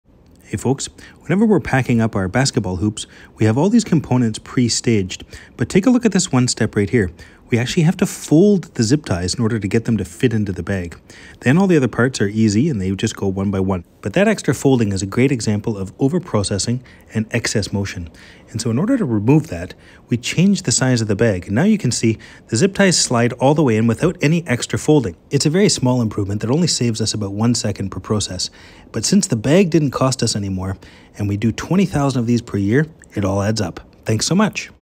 Streamlining Packing: Lean Improvement Saves Time and Effort!
Hi folks, In this before-and-after video, we implement a lean improvement to optimize our basketball hoop packing process. To remove the overprocessing and excess motion, we eliminate the unnecessary folding of zip ties to fit them into the bag. We streamline the process with a simple adjustment to the bag size, saving valuable time and resources.

What is LEAN? It’s a continuous improvement mindset that aims for excellence by simplifying processes, eliminating waste, and eliminating non-value-added activities.

[0206]